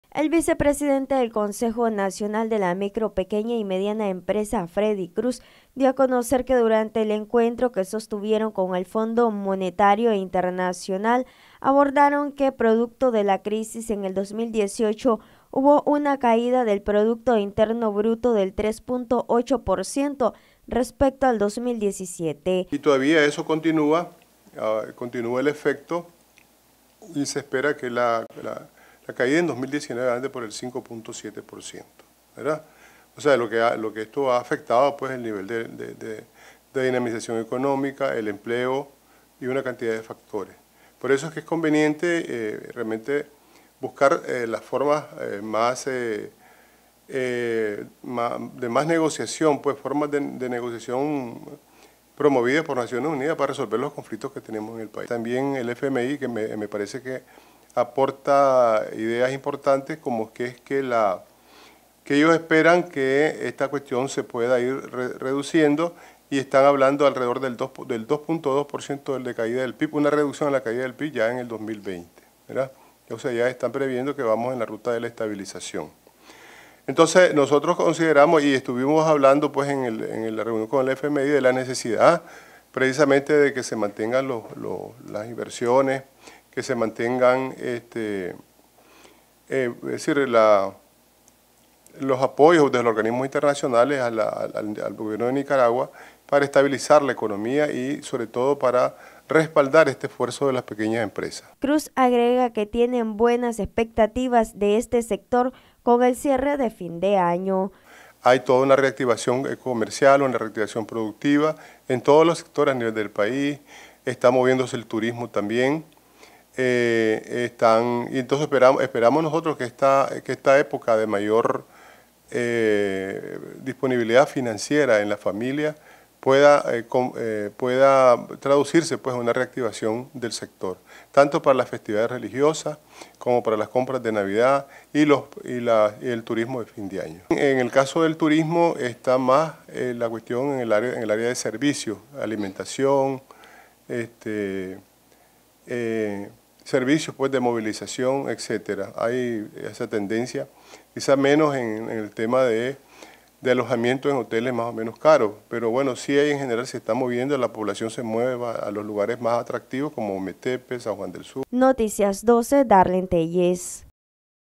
CRUZ: “SECTOR PYME ESPERAN TENER BUENOS RESULTADOS EN FIN DE AÑO”
Vice presidente de CONIMIPYME dio a conocer que durante la reunión que sostuvieron con representantes del FMI abordaron la situación que atraviesa dicho sector económico.